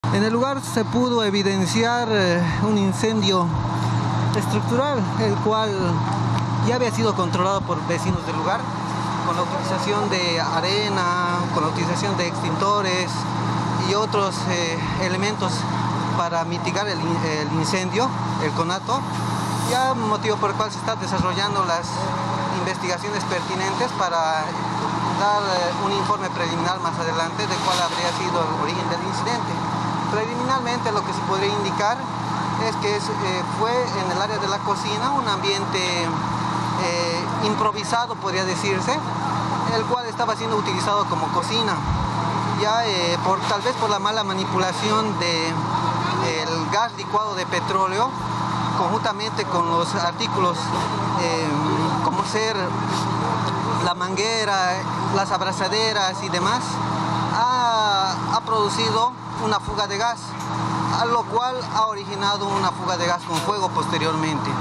Incendio en un restaurante
Bomberos reportaron un incendio en un restaurante en la doble vía la guardia, indagan las causas.